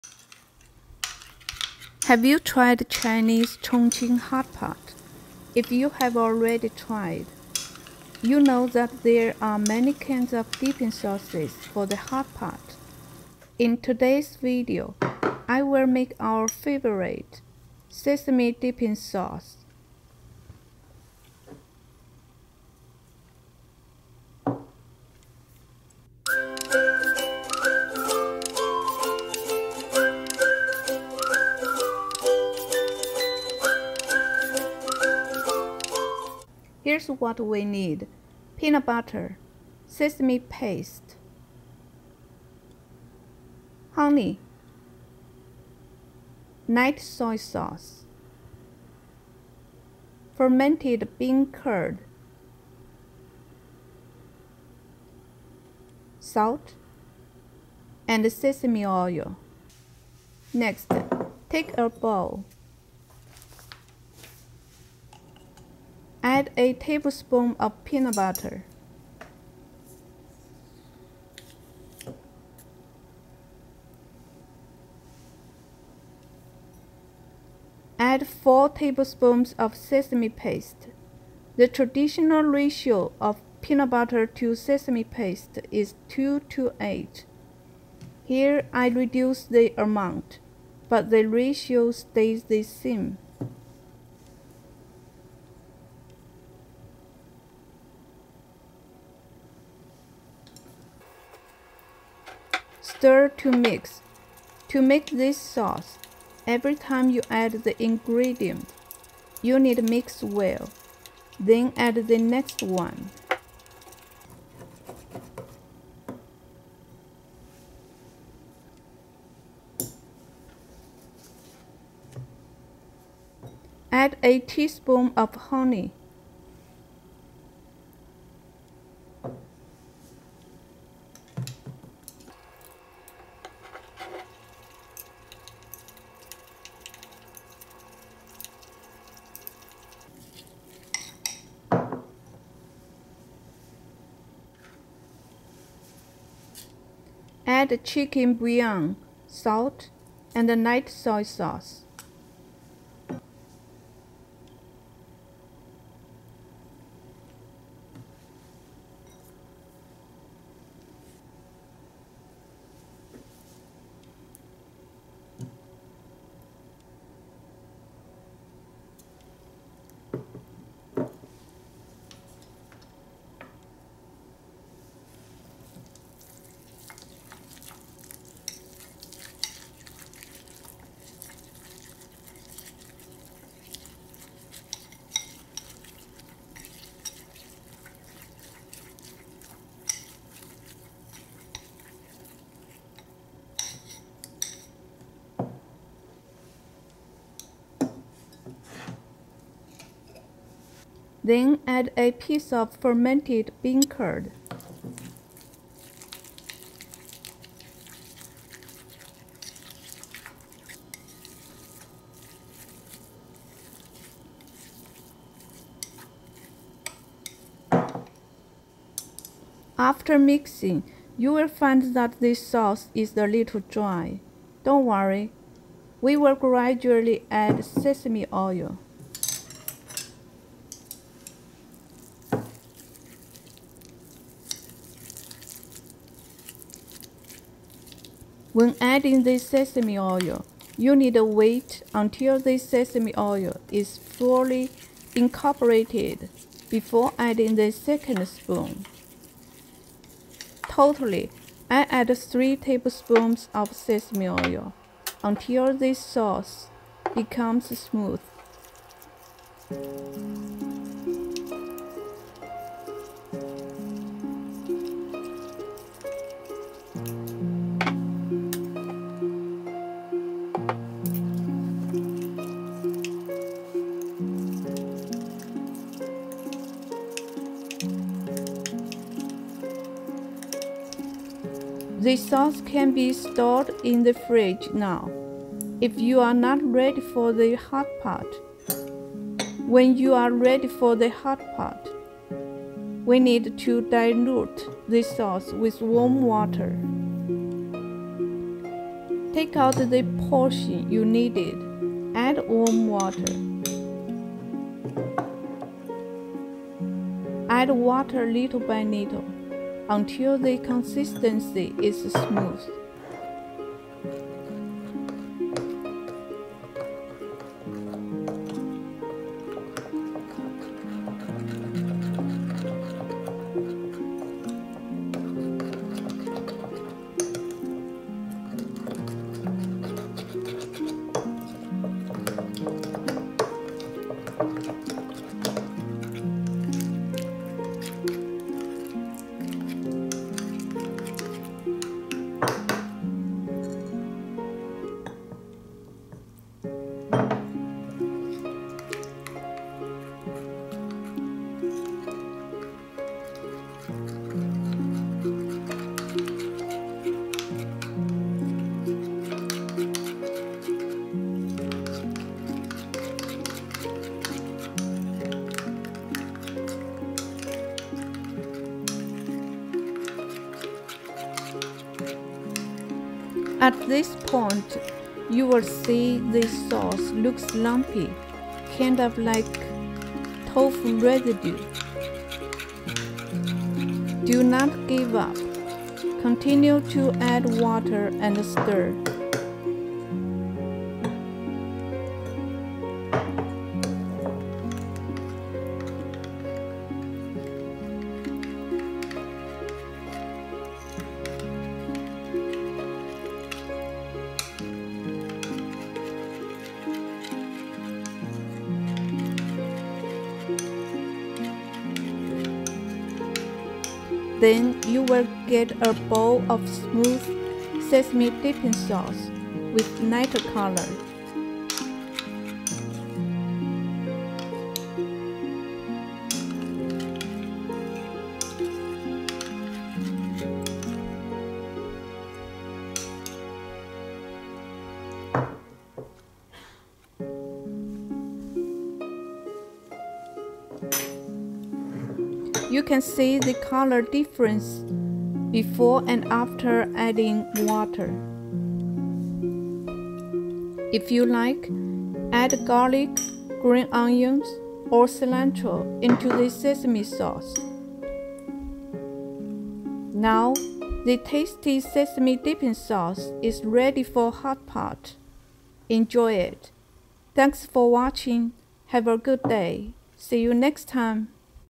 Hot Pot Sesame Dipping Sauce | 火锅芝麻蘸酱
Sesame Dipping Sauce for Hot Pot | 火锅芝麻蘸酱
Ingredients
1 tablespoon peanut butter 花生酱
4 tablespoons sesame paste 芝麻酱
1 teaspoon honey 蜂蜜
1/2 teaspoon chicken bouillon 鸡精
1/2 teaspoon salt 盐
1/2 teaspoon light soy sauce 生抽
1 fermented bean curd 豆腐乳
3 tablespoons sesame oil 芝麻油
water as needed 适量水